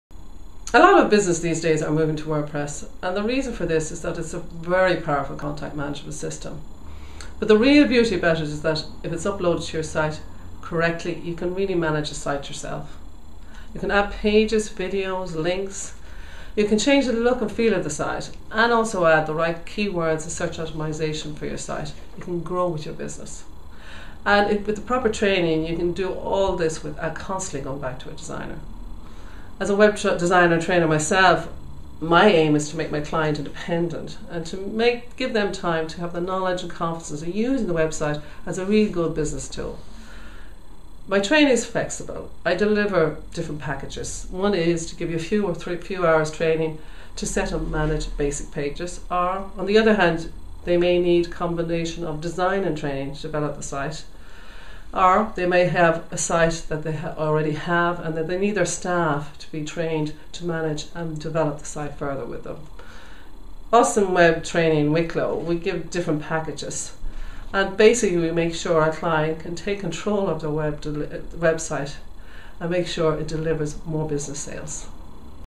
WordPress Training - Web Designer Dublin and Wicklow: wordpresstraining.ie
Sheena Gogarty an Irish web designer and trainer in WordPress software. When you create a website in WordPress software you have more control and you can develop the site to suit your business needs. covers Dublin, Wicklow and Wexford areas.